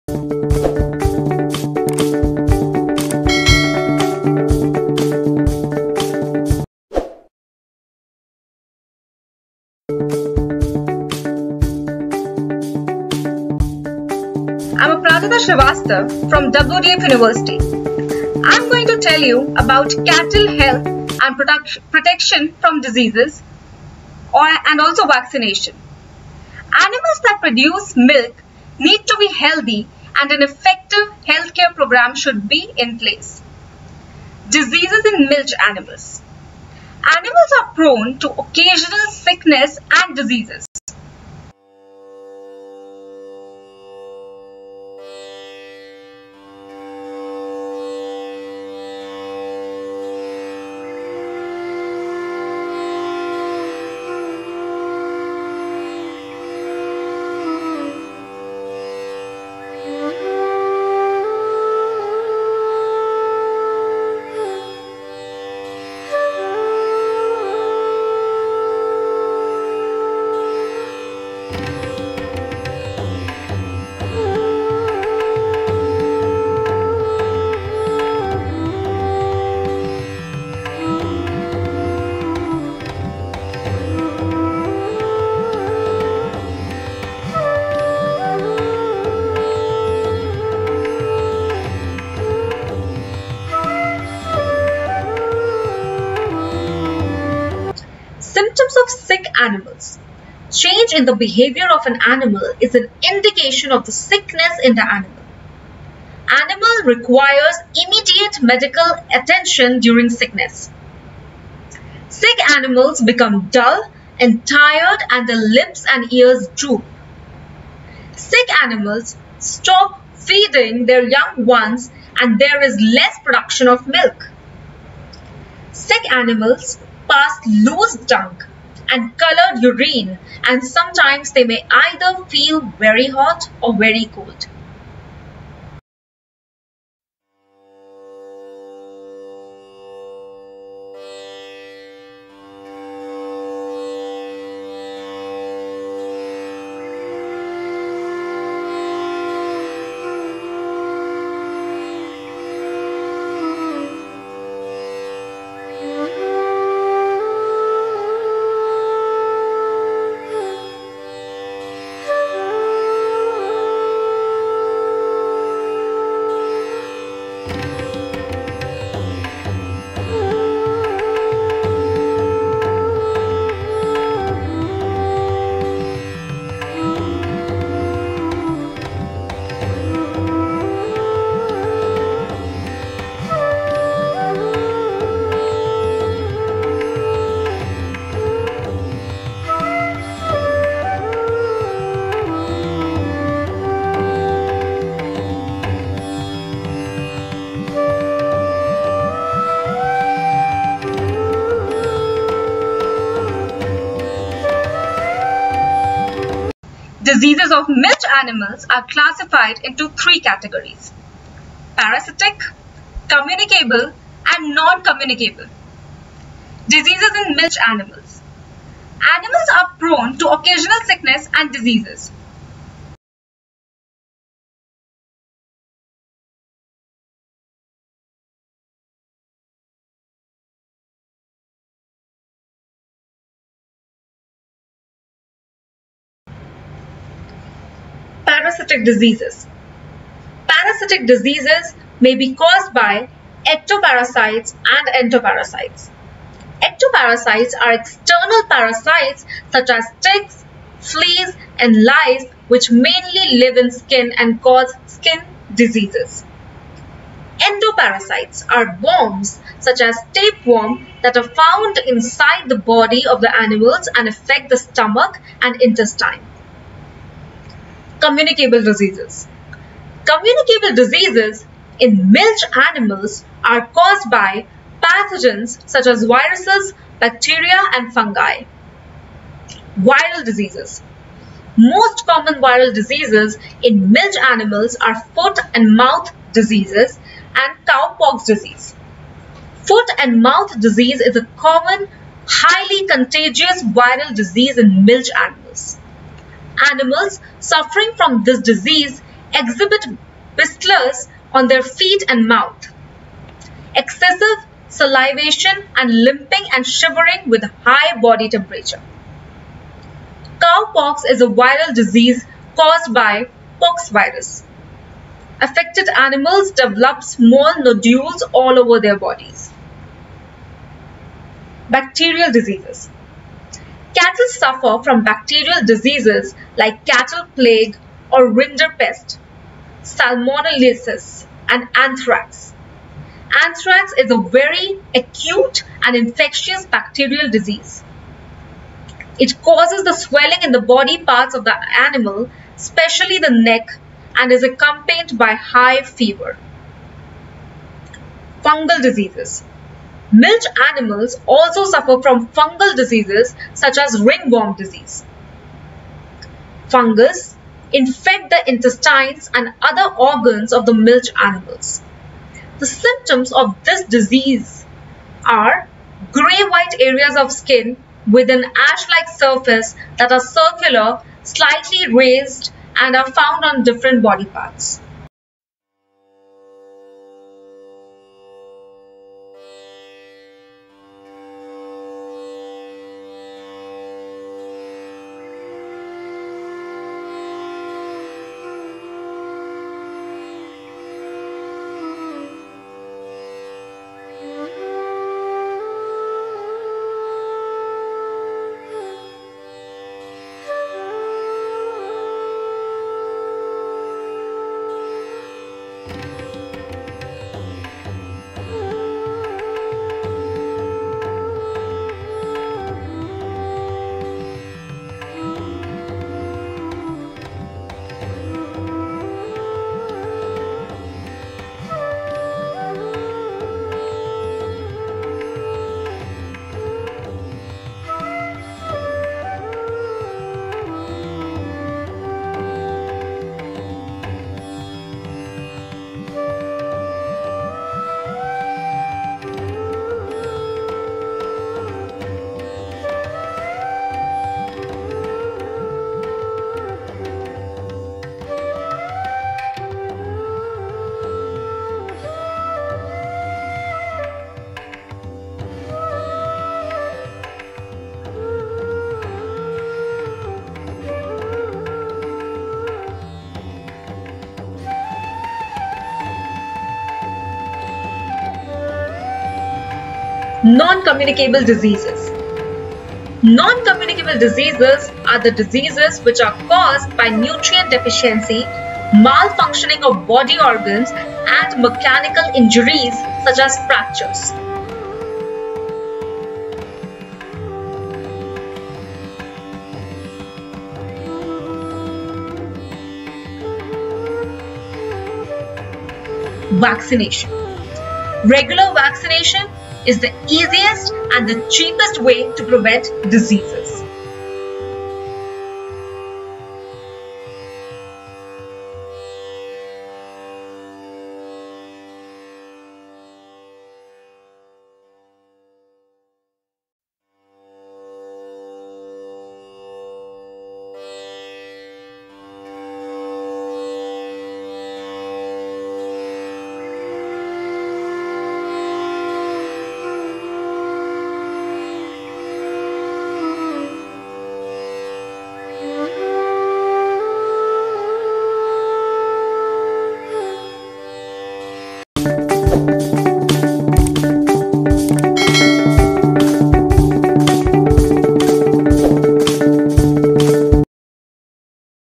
Cattle health and protection from diseases /Vaccination (In English)
Santé du bétail et protection contre les maladies / Vaccination
صحة الماشية وحمايتها من
الأمراض / التطعيم
Animals are prone to occasional sickness and diseases. Diseases of milch animals are classified into three categories: parasitic, communicable and non-communicable.
a) Parasitic diseases
Parasitic diseases may be caused by ectoparasites and endoparasites.
• Ectoparasites are external parasites such as ticks, fleas and lice which mainly live in skin and cause skin diseases.
• Endoparasites are worms such as tapeworm that are found inside the body of the animals and affect the stomach and intestine.
b) Communicable diseases
Communicable diseases in milch animals are caused by pathogens such as viruses, bacteria and fungi.
1) Viral diseases
Most common viral diseases in milch animals are foot and mouth diseases and cowpox disease. Foot and mouth disease is a common, highly contagious viral disease in milch animals. Animals suffering from this disease exhibit blisters on their feet and mouth, excessive salivation and limping and shivering with high body temperature.

• Cowpox is a viral disease caused by pox virus. Affected animals develop small nodules all over their bodies.

2) Bacterial diseases
Cattle suffer from bacterial diseases like cattle plague or rinderpest, salmonellosis and anthrax.
Anthrax is a very acute and infectious bacterial disease. It causes the swelling in the body parts of the animal, especially the neck, and is accompanied by high fever.
3) Fungal diseases
Milch animals also suffer from fungal diseases such as ringworm disease. Fungus infect the intestines and other organs of the milch animals. The symptoms of this disease are grey-white areas of skin with an ash-like surface that are circular, slightly raised and are found on different body parts.
c) Non-communicable diseases
Non-communicable diseases are the diseases which are caused by nutrient deficiency, malfunctioning of  body organs and mechanical injuries such as fractures
Vaccination
Regular vaccination is the easiest and cheapest way to prevent diseases.